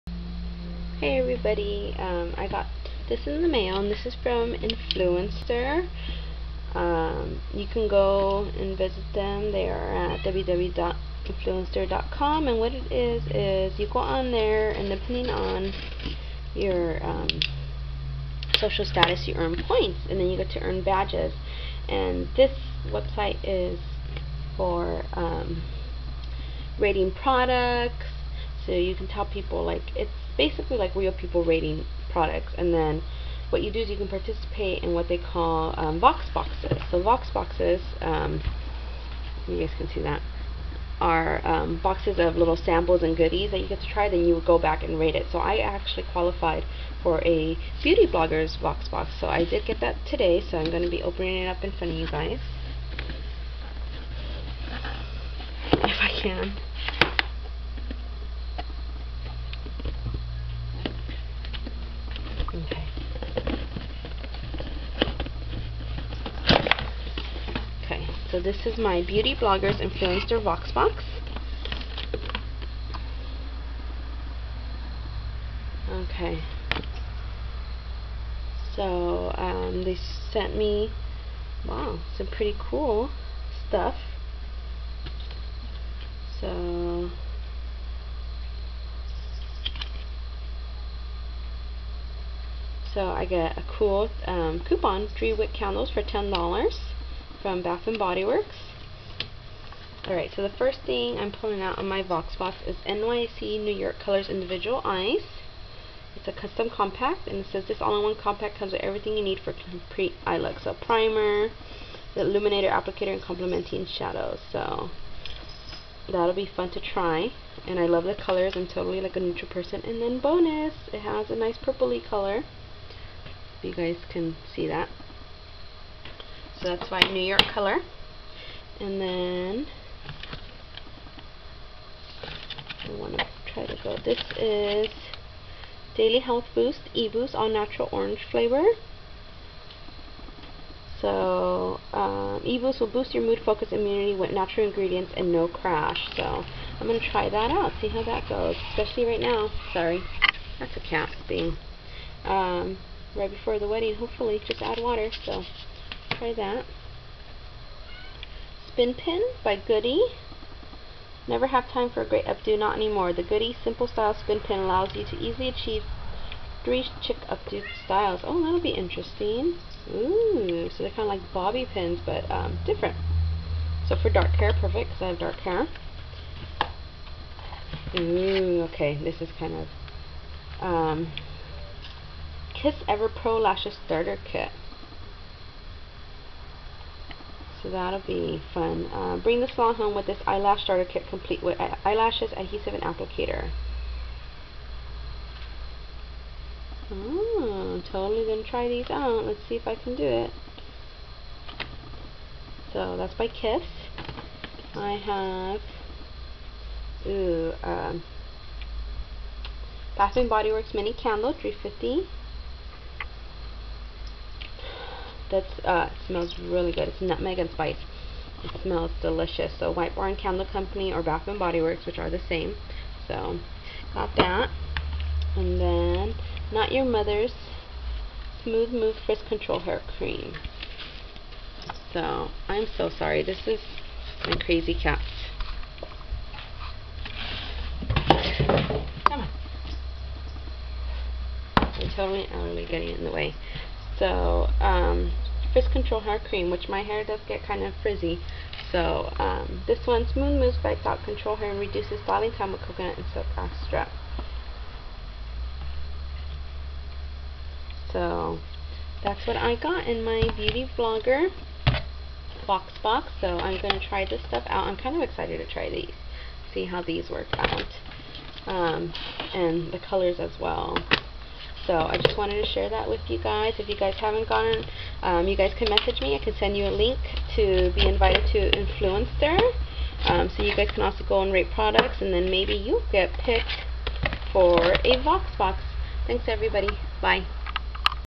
Influenster Vox Box
I am part of Influenster.com's Beauty Blogger Vox Box to tryout some great products then I rate and review each one. Here is the box they sent me. Check out influenster.com to see real reviews from people like you and me and to score some free stuff too!